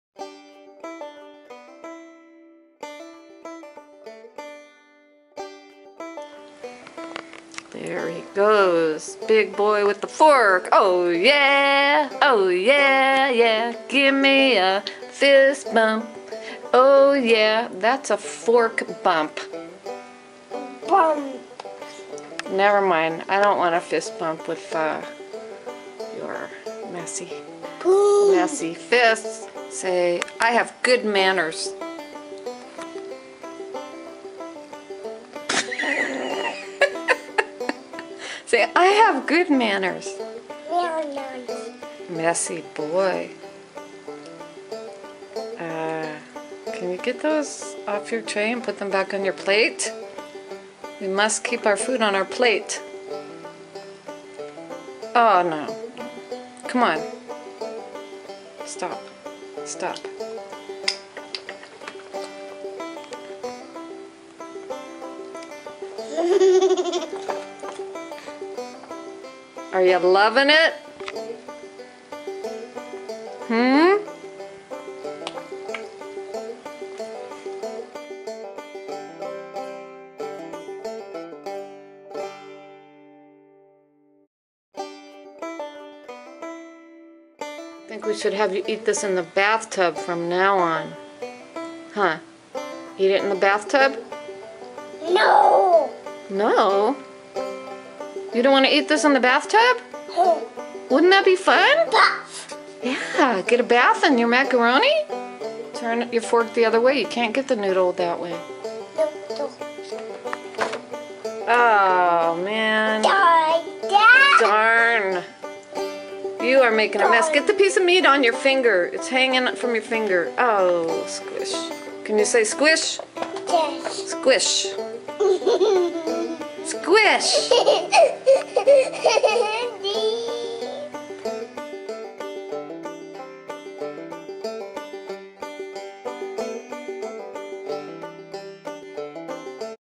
Baby's Macaroni Mania Part Two
Macaroni Mania Part Two
19-month-old baby finds eating beef macaroni with a fork challenging, so he got creative and had a blast. This was like edible finger paint to him. His parents don't let him feed himself beef macaroni, but Grammy didn't know. His parents weren't happy with his red hair or splatters on the wall. Grammy's fired!

Lynn Kelley author, children's author, Curse of the Double Digits, BBH McChiller, Monster Moon mysteries